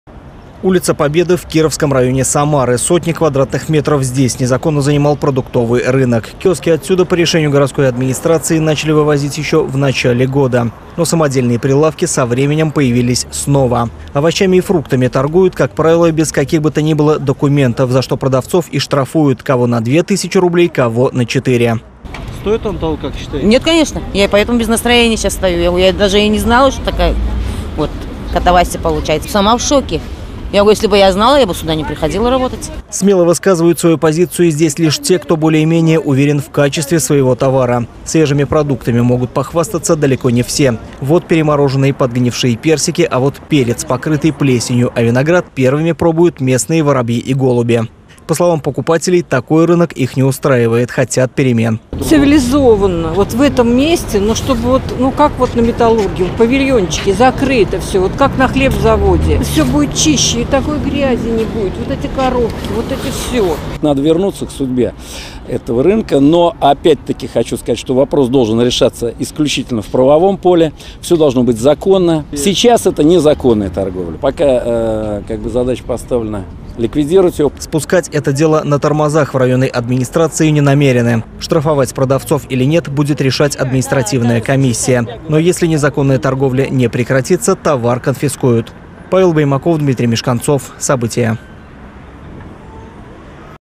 Лоточники продавали подгнившие фрукты и заплесневелые овощи
Освободить Самару от стихийных рынков. На улице Победы штрафовали лоточников, которые продавали товары сомнительного качества.